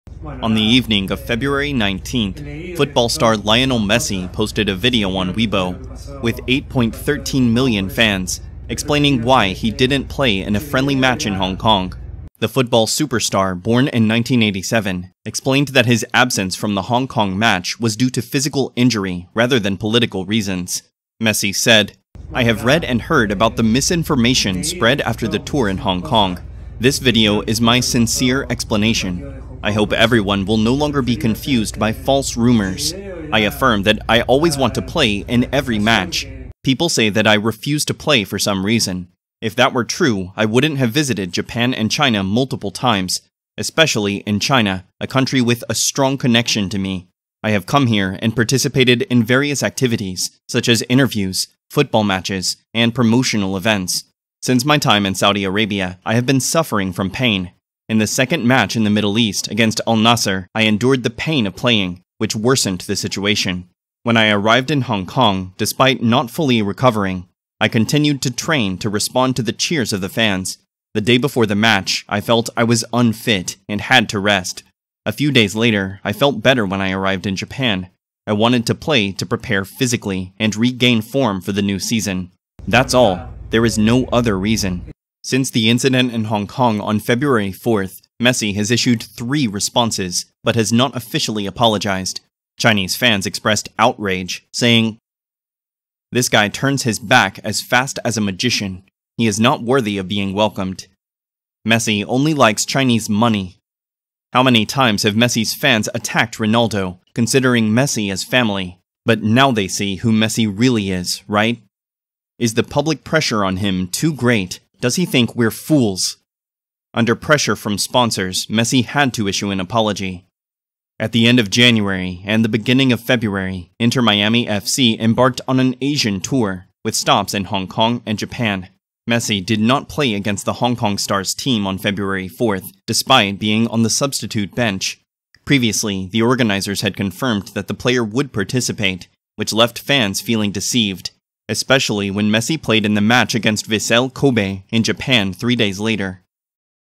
Messi posts video to calm fans, Chinese netizens still criticize
On the evening of February 19, football star Lionel Messi posted a video on Weibo, with 8.13 million fans, explaining why he did not play in a friendly match in Hong Kong. The football superstar born in 1987 dexplained that his absence from the Hong Kong match was due to physical injury rather than political reasons.